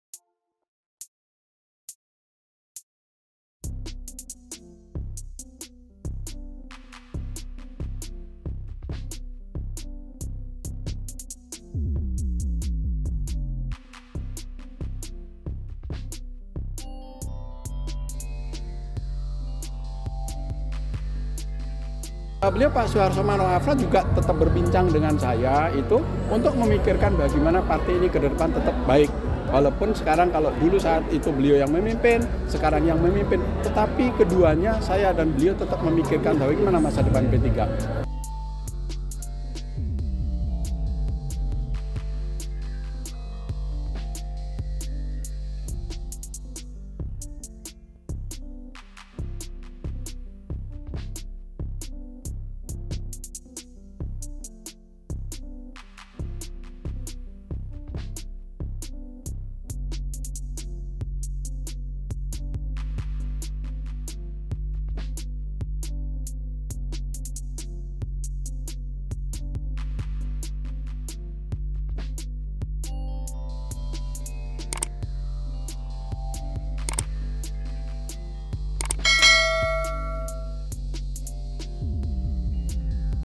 Mardiono PPP Akui Masih Jalin Hubungan Baik dengan Suharso Monoarfa
Plt Ketum Partai PPP M Mardiono buka suara soal nasib eks Ketum PPP Suharso Monoarfa. Suharso sendiri resmi dicopot dari jabatan Ketum PPP pada 30 Agustus 2022.

Meski begitu, Mardiono mengaki masih berhubungan baik dengan Suharso. Komunikasi keduanya masih terjalin hingga saat ini.

Penulis: Muhammad Naufal
Penulis Naskah:  Yohana Indah Nur Ratri
Video Editor: Adeline Frederica Simatupang
Produser: Okky Mahdi Yaser